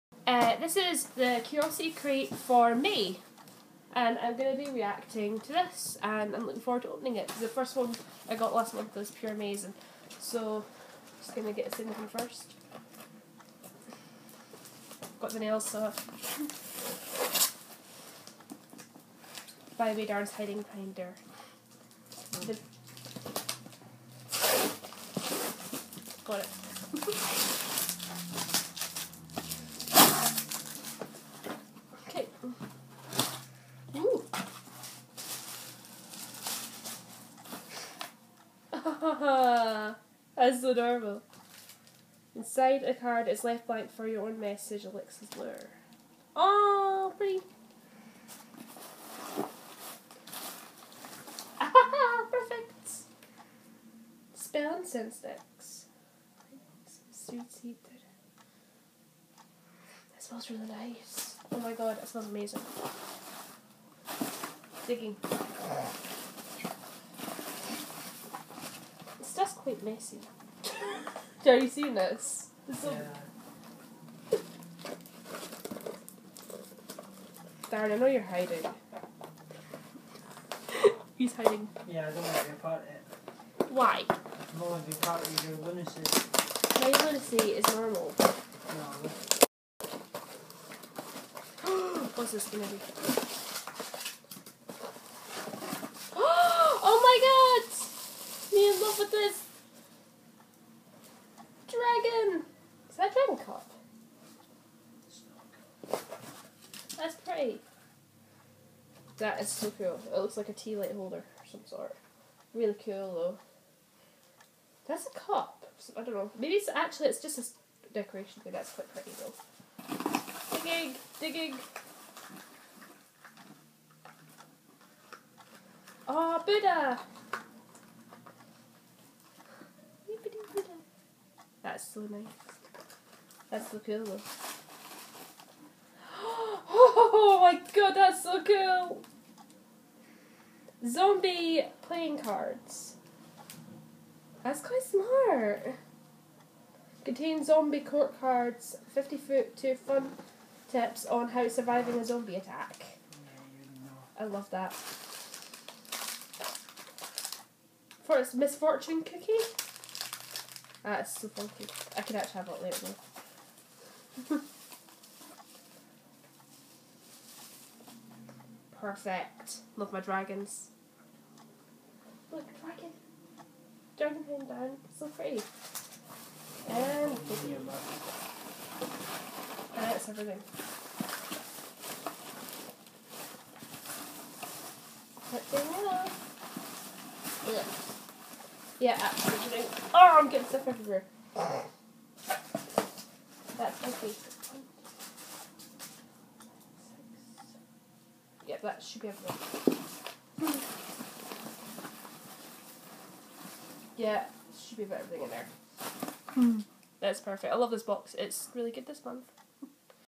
Curiosity Crate May Opening
I open my curiosity crate from Divinity in Stirling. Amazing stuff inside!